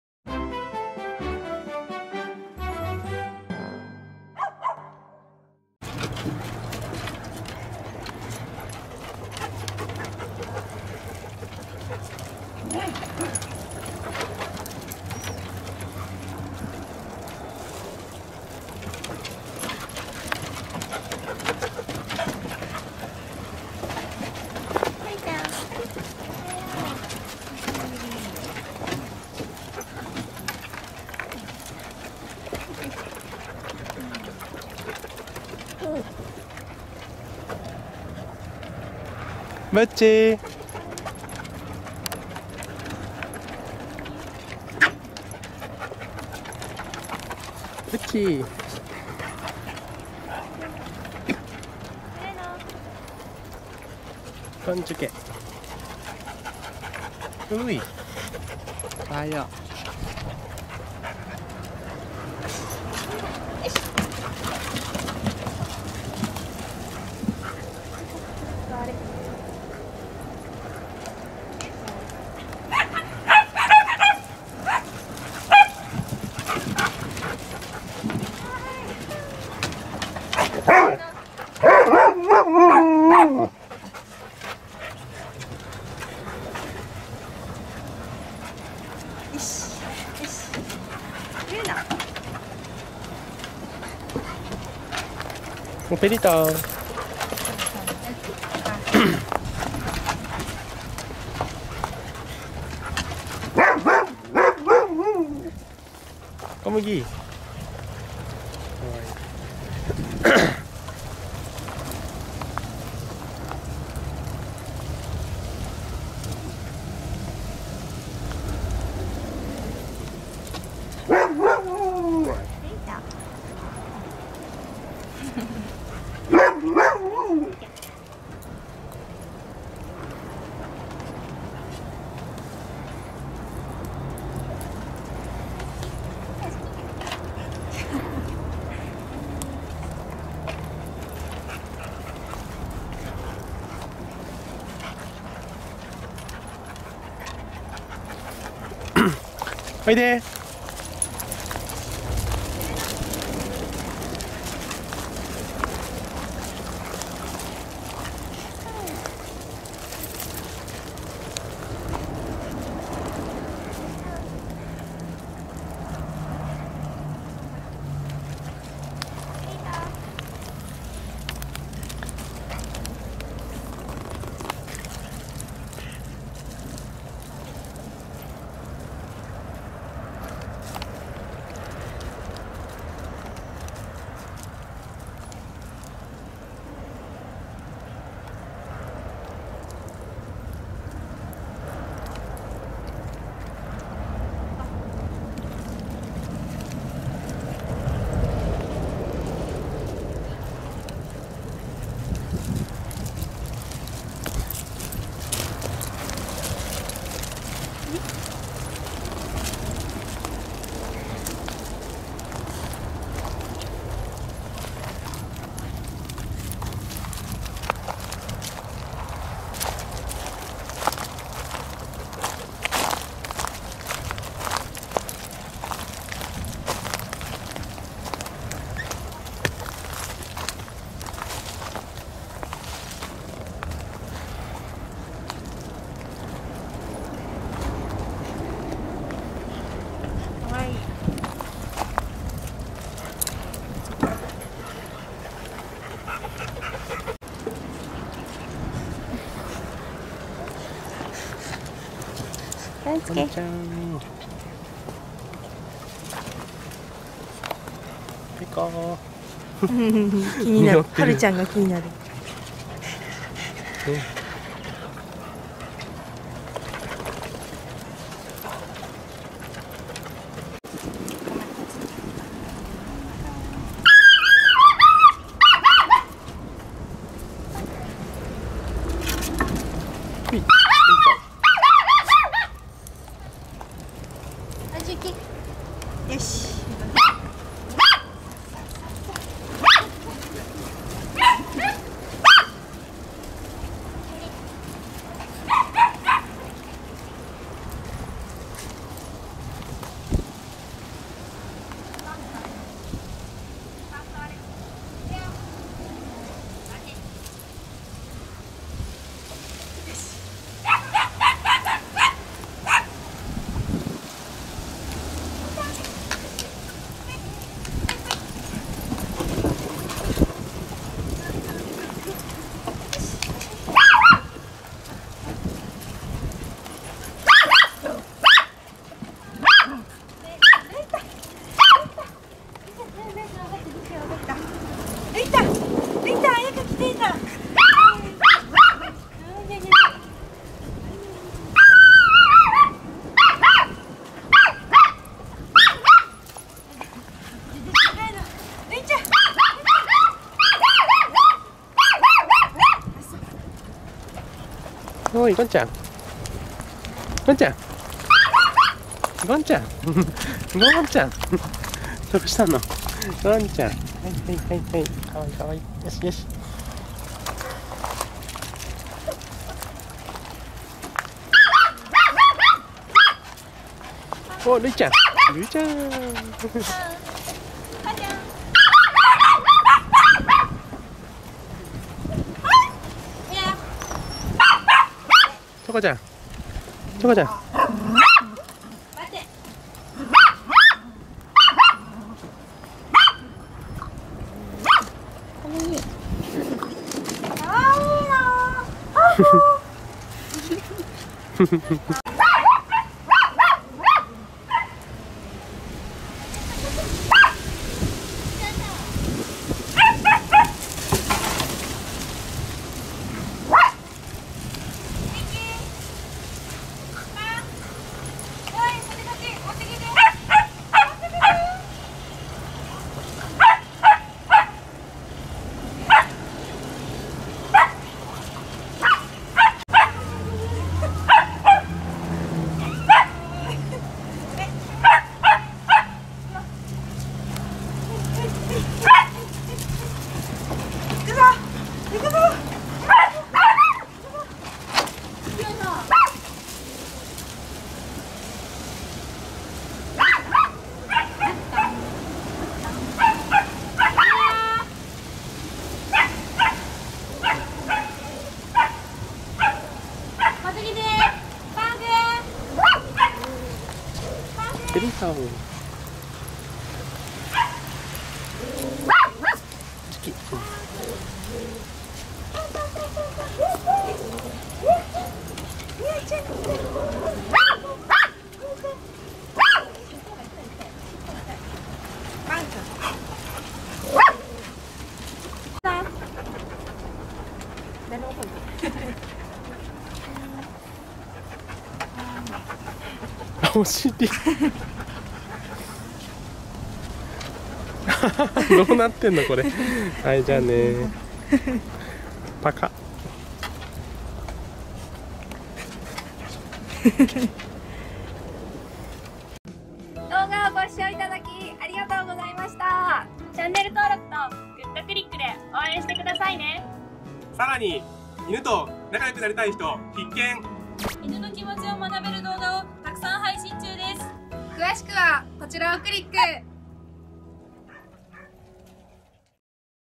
ペットホテル x 犬のしつけ 2020.12.13
ワンコ・ワークスの過去の人気動画をまとめてみました^^！

☆登場するのワンちゃんたちは、飼い主さん了承の元、YouTube上で公開されています☆

ワンコ・ワークスのペットホテル「おうちホテル」

ワンコ・ワークスのしつけ訓練